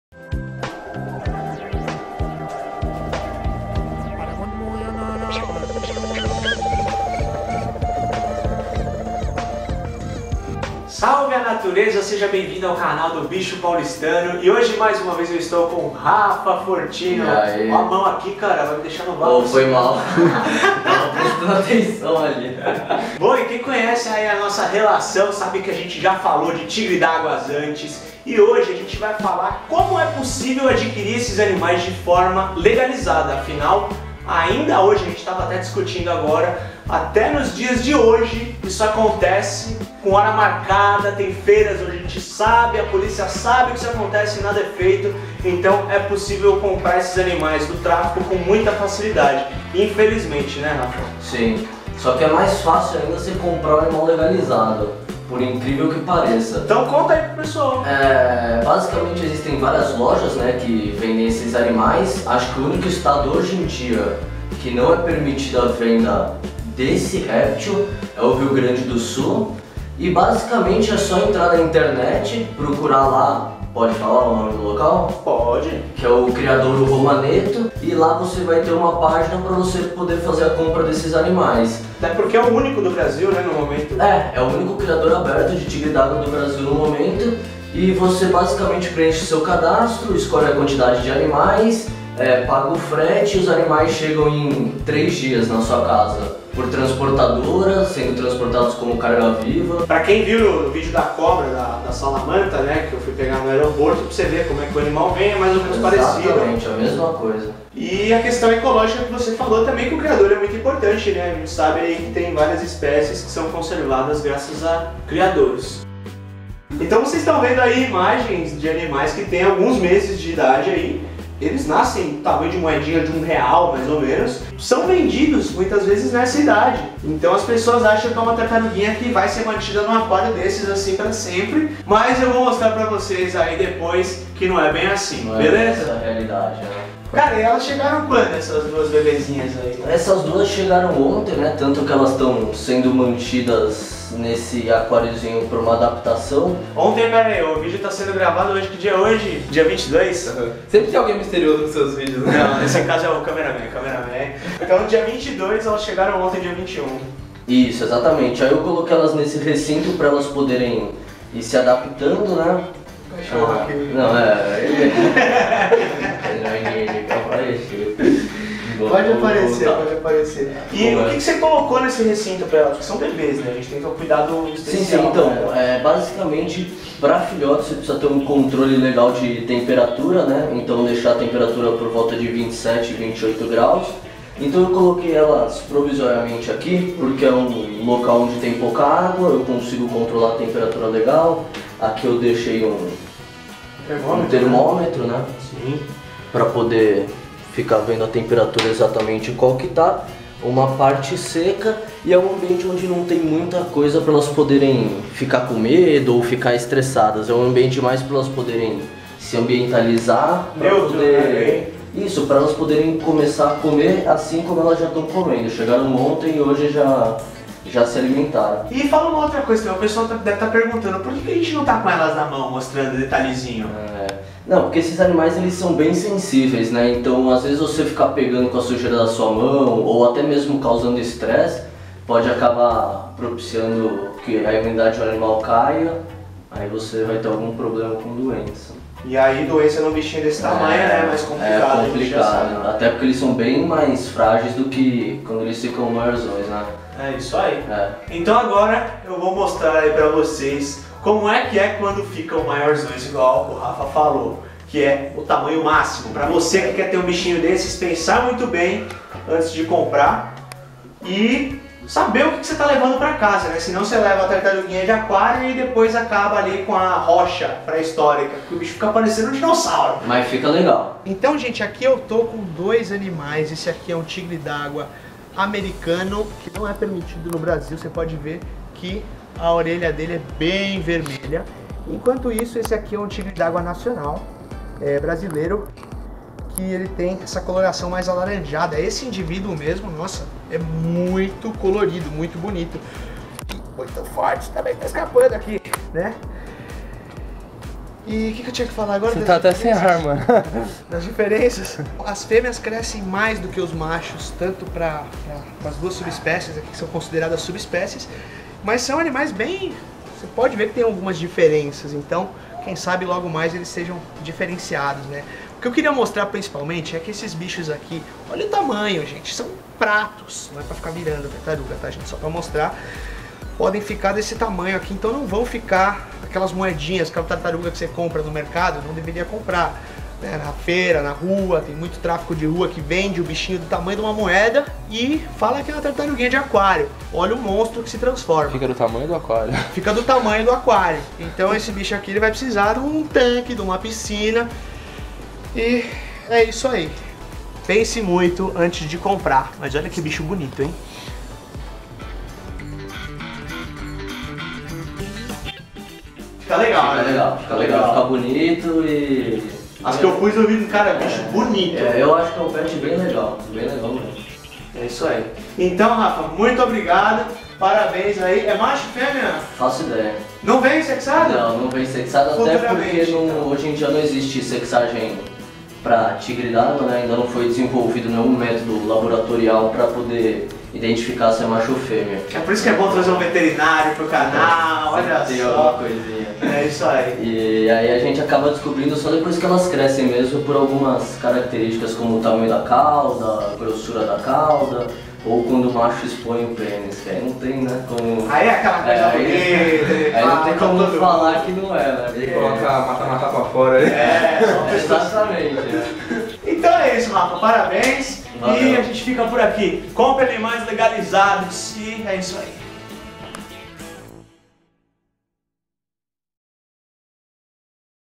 Adquirindo tartaruga Tigre D'Água LEGALIZADA! com Rapha Fortino
Aprenda neste vídeo como é possível adquirir uma tartaruga tigre-d'água (Trachemys dorbignyi) de forma correta, com o veterinário Rapha Fortino e participação também de Marcus Farah e do famoso câmera misterioso na produção.

Vídeo novo todas as quintas!
BP OFF todas as terças ímpares